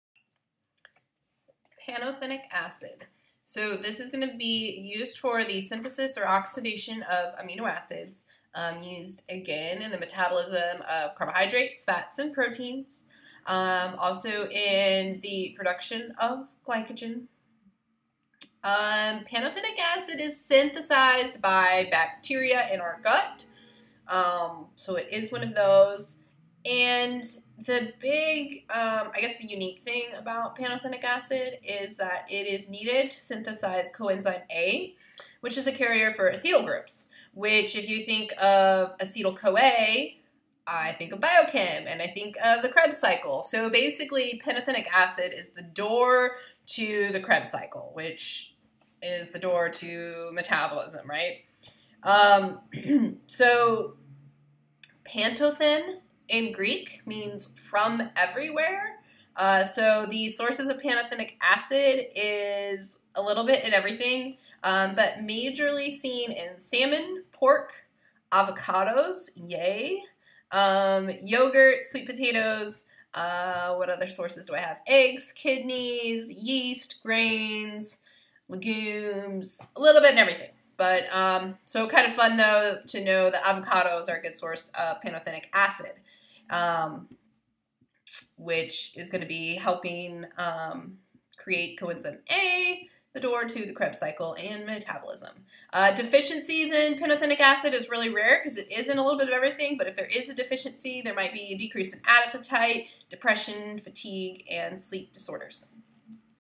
Domain 1 B5 Pantothenic Acid
Created by bacteria in intestine (along with vit K, B1, B3, B7)
Syn/oxidation of FATTY ACIDS, metabolism of CHO FAT PRO
Part of Coenzyme A "door to Krebs Cycle"
Sources: Animal protein, gains, legumes, eggs, kidney, live, avocado, salmon, yeast
S&S: Decreased appetite, depression, sleep disorders